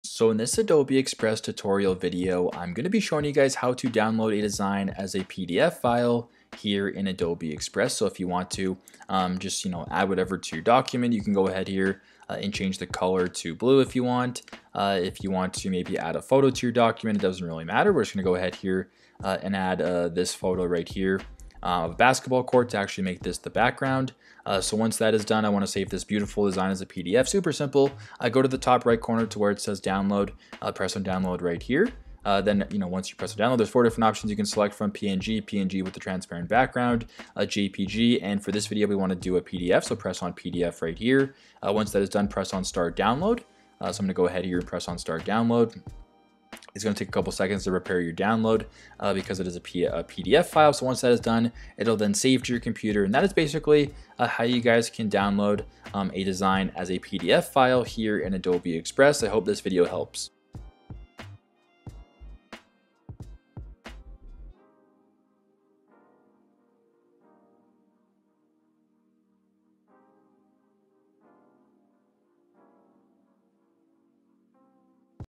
How To Download As A PDF File In Adobe Express | Adobe Express Tutorial
How To Download As A PDF File In Adobe Express | Adobe Express

Tutorial To download your design as a PDF file in Adobe Express, you can follow these simple steps:

Open Adobe Express: First, open the Adobe Express app on your device.

Create a new project: Click on the "+" button to create a new project or open an existing one.

Save your design: Once you have completed your design, click on the "Save" button located in the top right corner of the screen.

Choose PDF format: In the "Save" menu, select "PDF" from the available file format options.

Customize settings: You can now customize the settings for your PDF file, such as page orientation, size, and quality.

Save your design: Once you have customized the settings, click on the "Save" button to save your design as a PDF file.

Locate your file: Your PDF file will be saved in your device's default download location or the location you specified while saving the file.

That's it! You have successfully downloaded your design as a PDF file in Adobe Express. You can repeat these steps to download your design in other file formats or customize the settings for your PDF file as per your requirements.